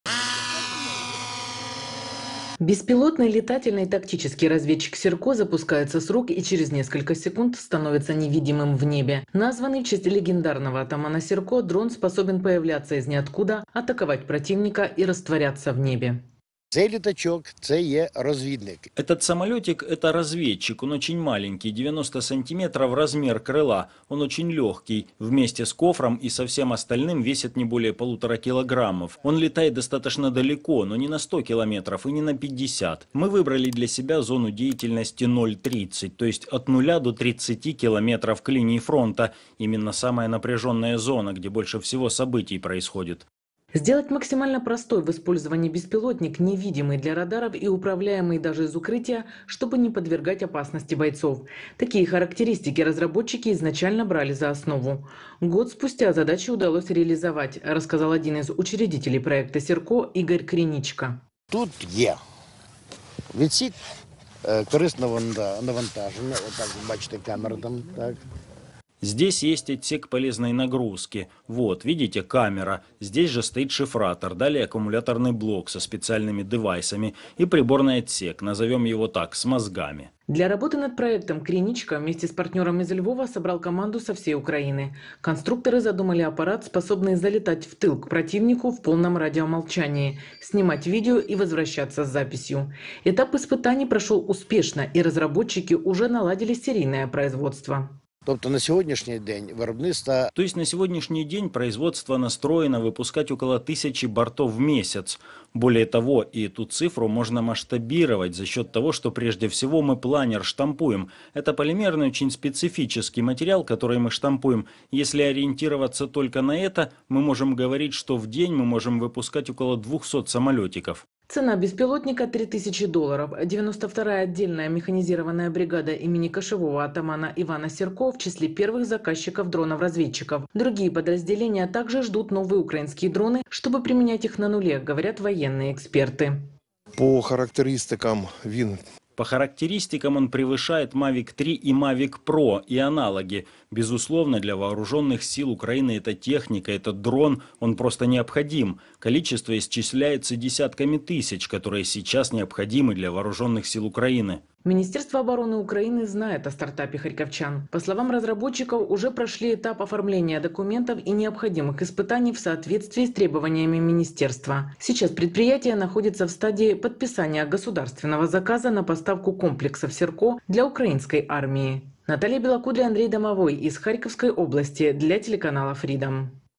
Харьковские конструкторы разработали беспилотник SIRKO
Накануне в Харькове был разработан беспилотник собственного производства под названием SIRKO. Он почти бесшумный и его тяжело распознать врагу. Как происходила разработка и как она сможет помогать ВСУ на фронте - смотрите в видео!